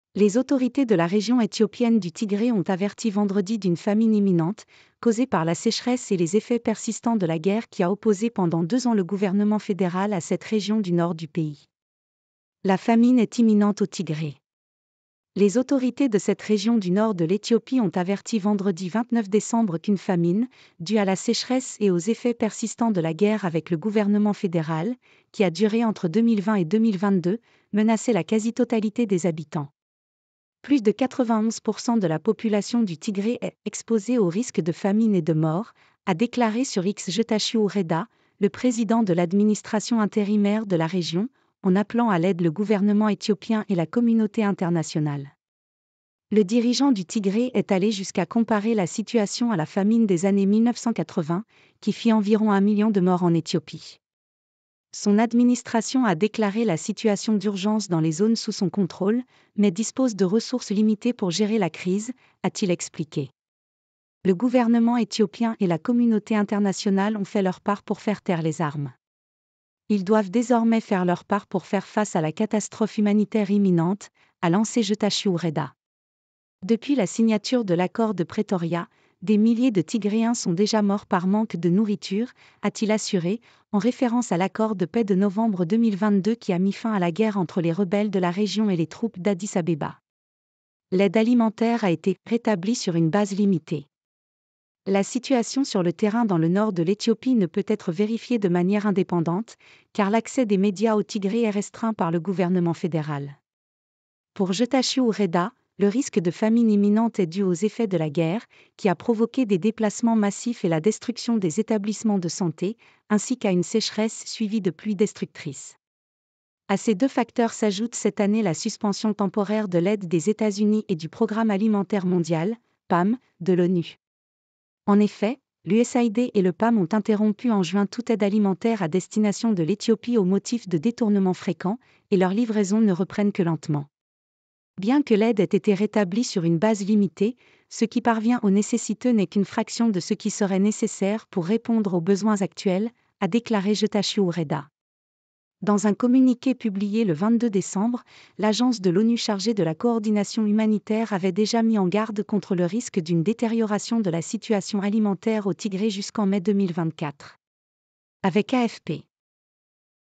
Famine imminente au Tigré, alerte des autorités éthiopiennes
Les autorités de la région éthiopienne du Tigré ont averti vendredi d'une famine imminente, causée par la sécheresse et les effets persistants de la guerre qui a opposé pendant deux ans le gouvernement fédéral à cette région du nord du pays.

La famine est imminente au Tigré.